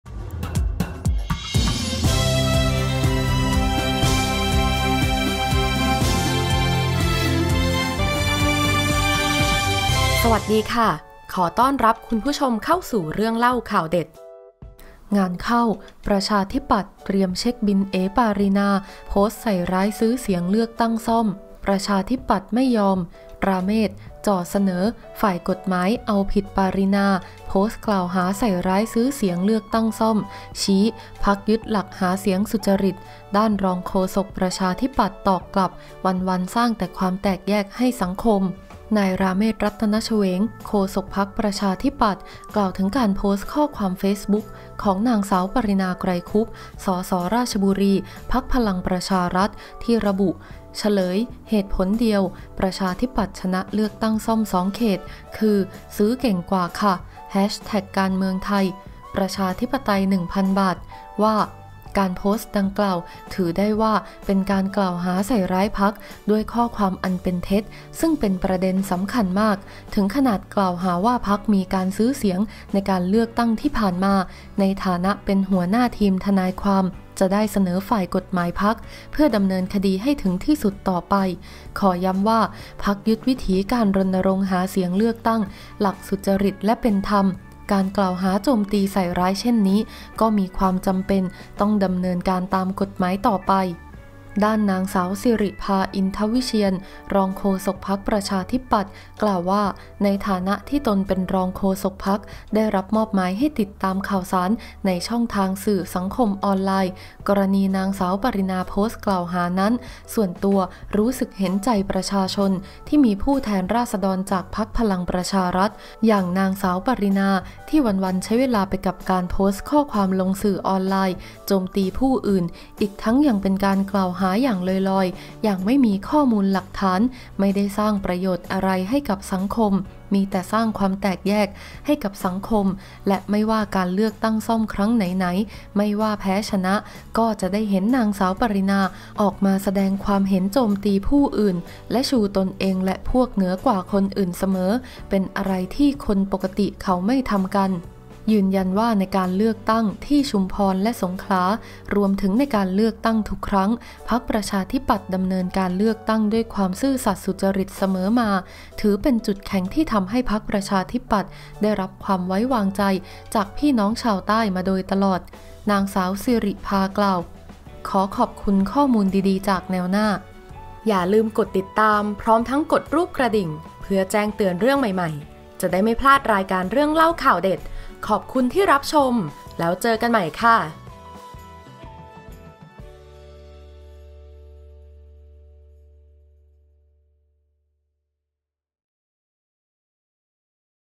อะอ้าว!งานเข้า "ประชาธิปัตย์" เตรียมเช็คบิล "เอ๋ ปารีณา" โพสต์ใส่ร้ายซื้อเสียงเลือกตั้งซ่อม
ปชป. ไม่ยอม! ‘ราเมศ’ จ่อเสนอฝ่ายกฎหมายเอาผิด ‘ปารีณา’ โพสต์กล่าวหาใส่ร้ายซื้อเสียงเลือกตั้งซ่อม ชี้ พรรคยึดหลักหาเสียงสุจริต ด้านรองโฆษกปชป. ตอกกลับ
วันๆสร้างแต่ความแตกแยกให้สังคม

นายราเมศ รัตนะเชวง โฆษกพรรคประชาธิปัตย์ (ปชป.) กล่าวถึงการโพสต์ข้อความเฟซบุ๊กของ น.ส.ปารีณา ไกรคุปต์ ส.ส.ราชบุรี พรรคพลังประชารัฐ (พปชร.) ที่ระบุ
“เฉลย เหตุผลเดียว ประชาธิปัตย์ชนะเลือกตั้งซ่อม 2 เขต คือ ซื้อเก่งกว่าค่ะ
ประชาธิปไตย1000บาด” ว่า

การโพสต์ดังกล่าวถือได้ว่า เป็นการกล่าวหาใส่ร้ายพรรคด้วยข้อความอันเป็นเท็จ ซึ่งเป็นประเด็นสำคัญมาก ถึงขนาดกล่าวหาว่าพรรคมีการซื้อเสียงในการเลือกตั้งที่ผ่านมา
ในฐานะเป็นหัวหน้าทีมทนายความ จะได้เสนอฝ่ายกฎหมายพรรคเพื่อดำเนินคดีให้ถึงที่สุดต่อไป ขอย้ำว่า พรรคยึดวิถีการรณรงค์หาเสียงเลือกตั้งหลักสุจริตและเป็นธรรม การกล่าวหาโจมตีใส่ร้ายเช่นนี้
ก็มีความจำเป็นต้องดำเนินการตามกฎหมายต่อไป

ด้าน น.ส.ศิริภา อินทวิเชียร รองโฆษกพรรคประชาธิปัตย์ กล่าวว่า ในฐานะที่ตนเป็นรองโฆษกพรรค ได้รับมอบหมายให้ติดตามข่าวสารในช่องทางสื่อสังคมออนไลน์ กรณี น.ส.ปารีณาโพสต์กล่าวหานั้น
ส่วนตัวรู้สึกเห็นใจประชาชน ที่มีผู้แทนราษฎรจากพรรคพลังประชารัฐ อย่าง น.ส.ปารีณา ที่วันๆใช้เวลาไปกับการโพสต์ข้อความลงสื่อออนไลน์ โจมตีผู้อื่น อีกทั้งยังเป็นการกล่าวหาอย่างลอยๆ
อย่างไม่มีข้อมูลหลักฐาน ที่ไม่ได้สร้างประโยชน์อะไรให้กับสังคม มีแต่สร้างความแตกแยกให้กับสังคม และไม่ว่าการเลือกตั้งซ่อมครั้งไหนๆ ไม่ว่าแพ้ชนะ ก็จะได้เห็น น.ส.ปารีณาออกมาแสดงกความเห็นโจมตีผู้อื่น
และชูตนเองและพวกเหนือกว่าคนอื่นเสมอ เป็นอะไรที่คนปกติเขาไม่ทำกัน

“ยืนยันว่าในการเลือกตั้งที่ชุมพรและสงขลา รวมถึงในการเลือกตั้งทุกครั้ง พรรคประชาธิปัตย์ดำเนินการเลือกตั้งด้วยความซื่อสัตย์สุจริตเสมอมา ซึ่งถือเป็นจุดแข็งที่ทำให้พรรคประชาธิปัตย์
ได้รับความไว้วางใจจากพี่น้องชาวใต้มาโดยตลอด” น.ส.ศิริภา กล่าว

ขอขอบคุณข้อมูลดีๆจาก แนวหน้า